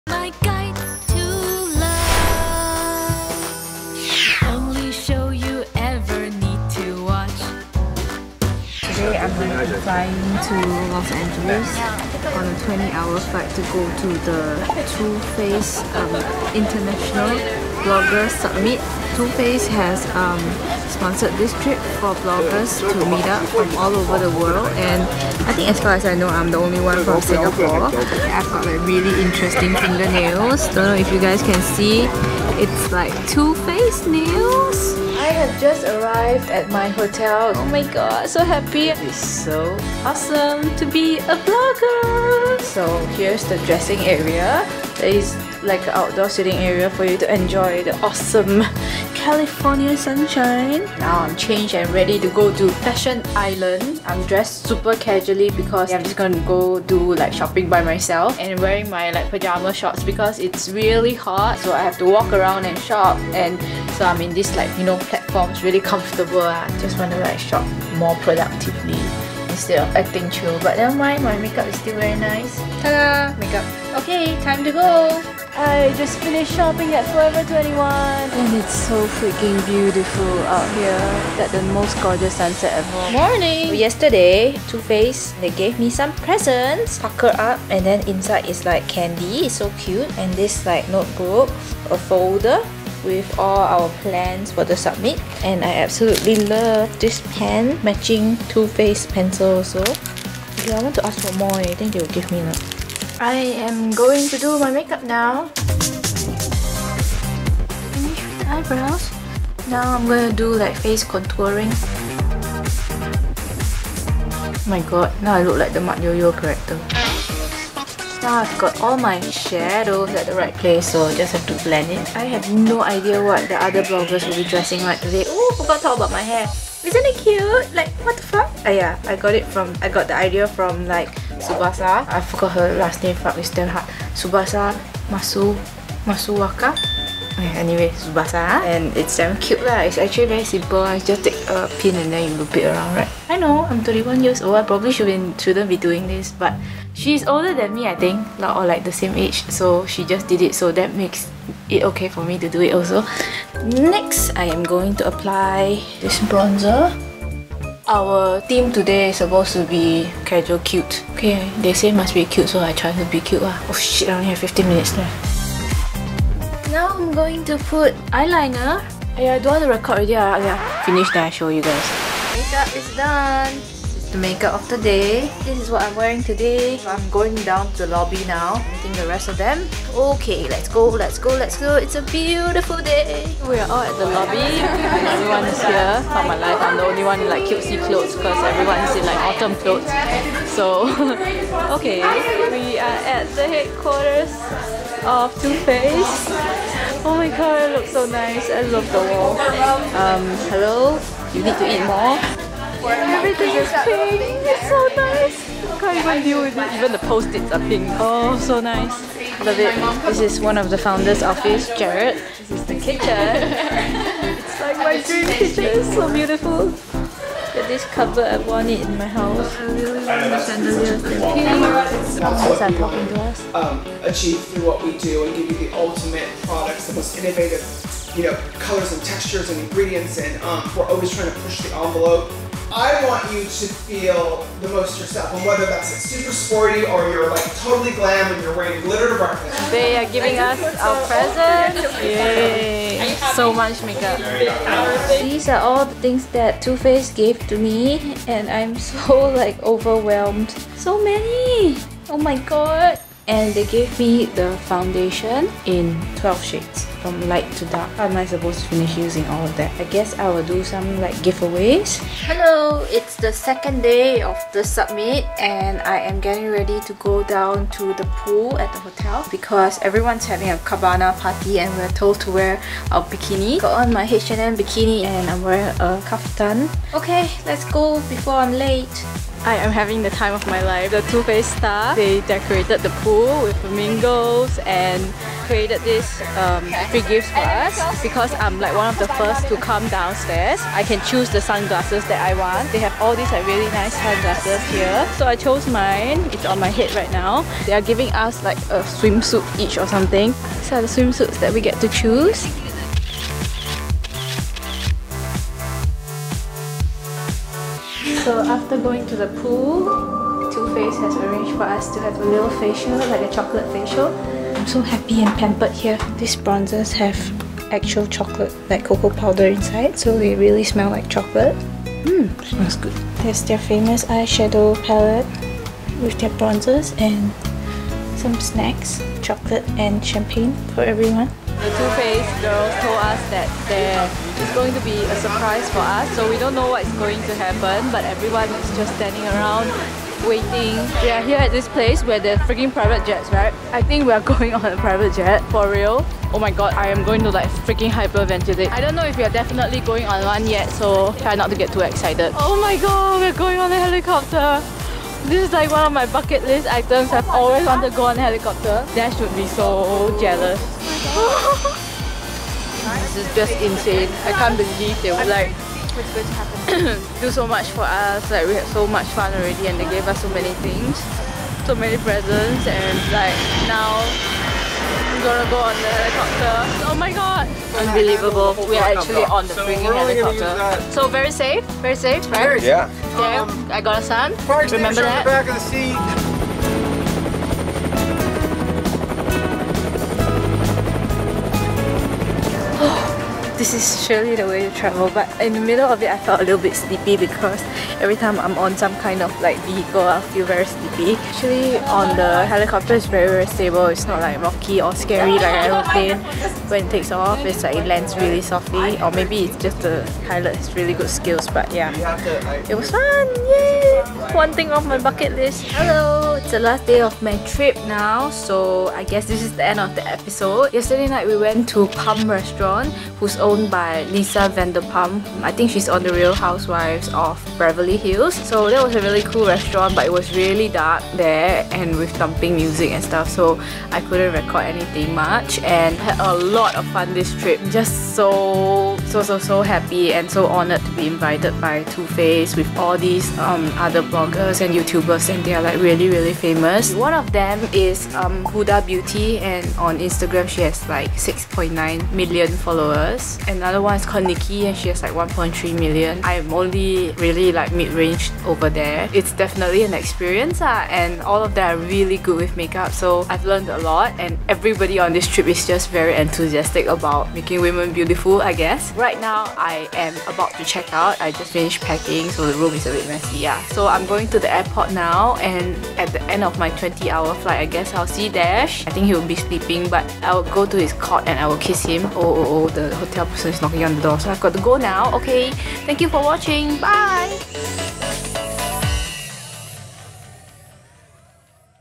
My L.A. Trip Vlog - Xiaxue's Guide To Life: EP173
A pool party with famous beauty bloggers, a chocolate spa, and a helicopter ride? Just a few highlights from Wendy’s recent L.A. trip!